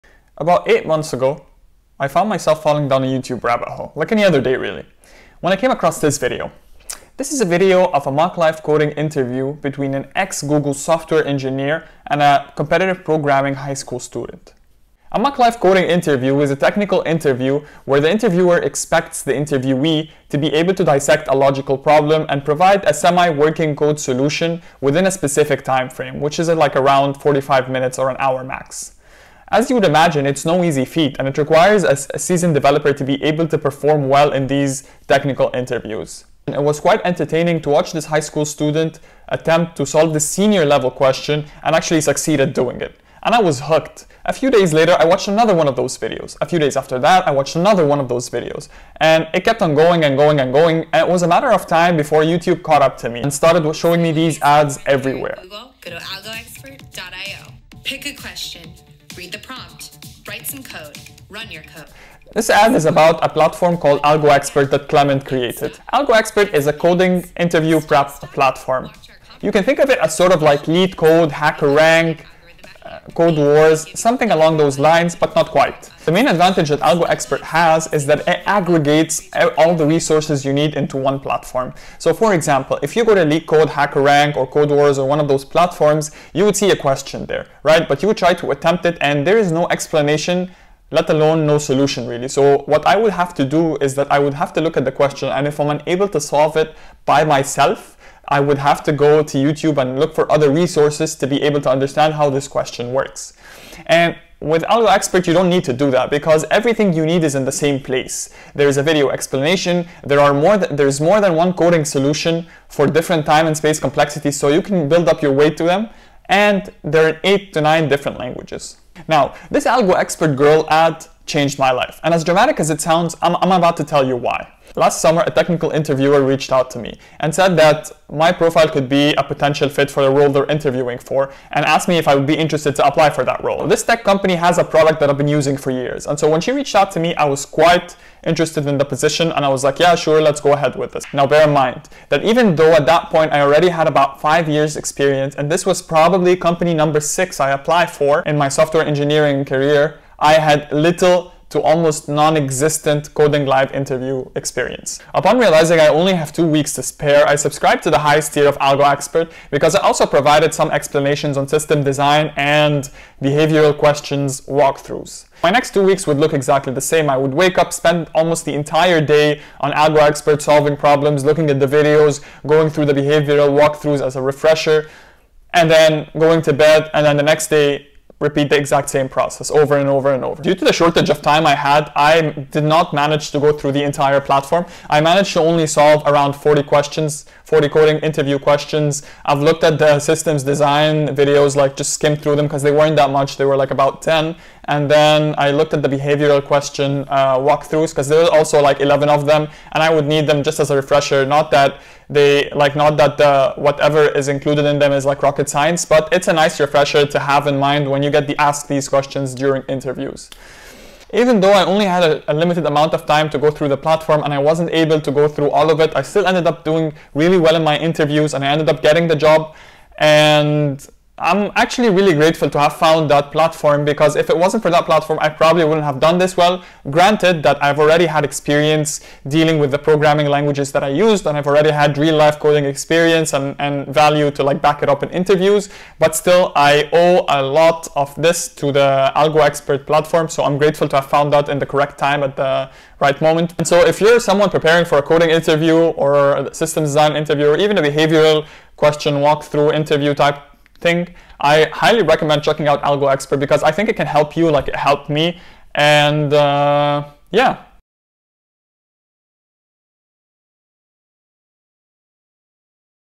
Why I love AlgoExpert ? | Coding Interview Prep Advice
This is a video about my personal journey with Algoexpert.io
I go on to describe how I came across Algoexpert.io and how that discovery had a profound effect on the trajectory of my career